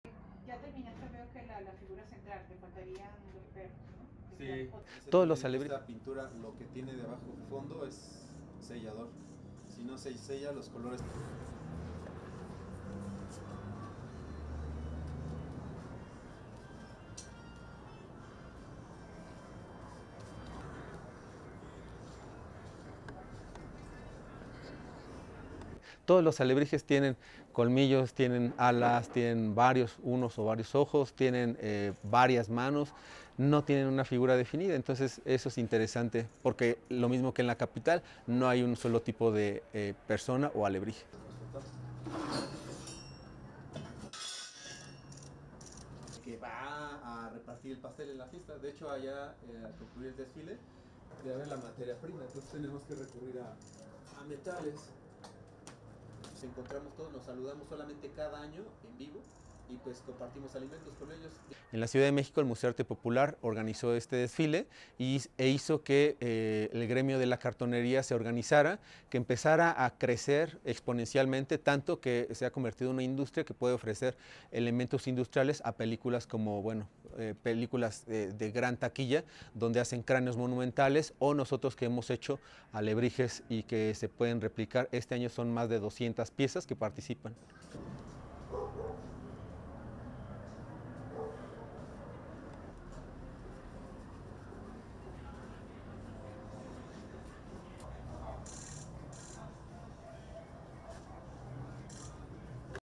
Mexico artist makes marvelous creatures
ASSOCIATED PRESS
Mexico City, Mexico - 19 October 2023
HEADLINE: Mexico artist makes marvelous creatures
1. Setup of papier-mâché artisan Israel Mondragón
2. Various of Mondragón painting papier-mâché creation
ANNOTATION: Artists are building fantastical creatures, known as 'alebrijes', to adorn Mexico City's central avenue as part of the Day of the Dead celebrations.
ANNOTATION: To make the sculptures, papier-mâché is layered on top of a wire structure and painted in bright colors.
3. SOUNDBITE (Spanish) Israel Mondragón, papier-mâché artisan:
"All the alebrijes have fangs, have wings, have one or several eyes, have several hands, but they don't have a defined figure. That's interesting because just like in Mexico City, there is not only one type of person or alebrije."
4. Various of Mondragón assembling his creation
ANNOTATION: The origins of the 'alebrijes' is unclear - it is believed a cardboard maker in the capital began the trend which was upheld by his family.
ANNOTATION: They are an expression of Mexican folk art that has become all the more popular thanks to the animated movie 'Coco'.
5. Mouth of Mondragón's creation
6. SOUNDBITE (Spanish) Israel Mondragón, Papier-mâché Artisan:
"In Mexico City the Museum of Popular Art organized a parade and made the cardboard guild get organized, which began to grow exponentially, so much so that it has become an industry that can offer elements to major box office films where they make use of huge skulls or we make alebrijes that can be replicated. This year there are more than 200 pieces participating."
7. Tilt down of Mondragón's creation
ANNOTATION: Mondragón's creation will join hundreds of other colorful creatures which will prowl along Mexico City's central avenue.
8. Various of Mondragón painting his creation
STORYLINE:
In the heart of Mexico City - strange and fabulous creatures have arrived.

One of the artists responsible for the surrealist creations is Israel Mondragón.

As he paints, a giant figure with the head of an iguana and body of a lizard occupies the patio of his house in a working-class neighborhood in the south of the Mexican capital.

It takes most of the day to finish his two-meter high surrealist creation, a work that will be part of the 200 large-scale pieces to be exhibited on one of Mexico City’s main avenues.

The “alebrijes,” are figures made of paper or cardboard with a wire structure. They are an expression of Mexican folk art that has become all the more popular thanks to the Pixar film Coco.

For more than a decade, the Museum of Popular Art has contributed to promote the artistic branch of the "cartoneros" or "alebrijeros", as the artisans who make alebrijes are usually called.

It is not clear how it started, but specialists identify Pedro Linares López, a cardboard maker from Mexico City, as its initiator, who around 1936 began to make the first alebrijes, a tradition that has been carried on by his children and grandchildren.

Mondragón still gets emotional when talking about showcasing his work to hundreds of spectators.

His creation, the "Señor Travieso,” depicts an iguana walking on two legs while carrying a cake with his left hand and walking three dogs.

Modragón came up with the design a year ago to promote the adoption of stray animals.

As he puts the last brushstrokes on the tail of "Señor Travieso,” the artist can’t hide the pride to have his alebrije amongst hundreds in Mexico City this year.

AP Video shot by Gerado Carillo